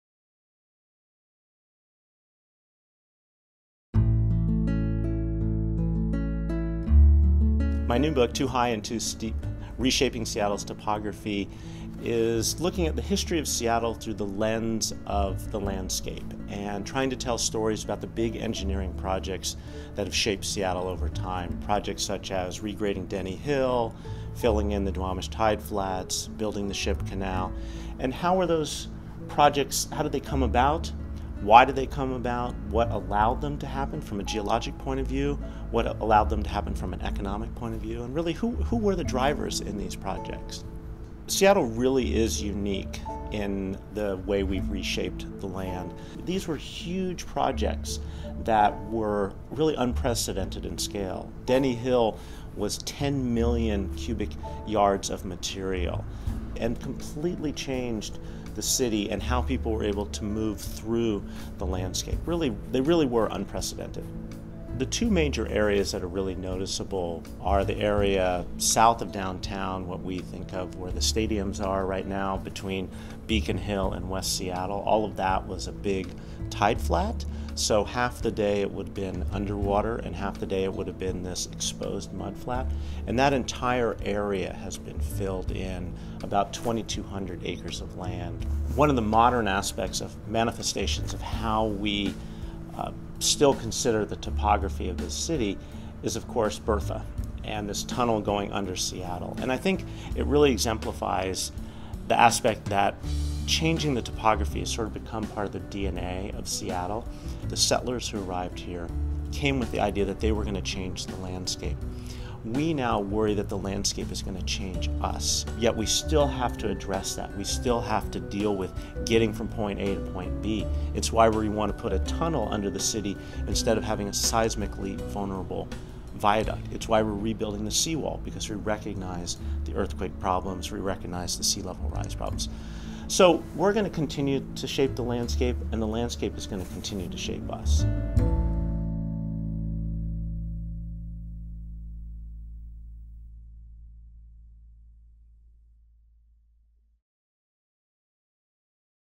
Too High and Too Steep: Reshaping Seattle Topography
*Forthcoming September 2015*
Residents and visitors in today’s Seattle would barely recognize the landscape that its founding settlers first encountered. As the city grew, its leaders and inhabitants dramatically altered its topography to accommodate their changing visions. In Too High and Too Steep, David B. Williams uses his deep knowledge of Seattle, scientific background, and extensive research and interviews to illuminate the physical challenges and sometimes startling hubris of these large-scale transformations, from the filling in of the Duwamish tideflats to the massive regrading project that pared down Denny Hill.

In the course of telling this fascinating story, Williams helps readers find visible traces of the city’s former landscape and better understand Seattle as a place that has been radically reshaped.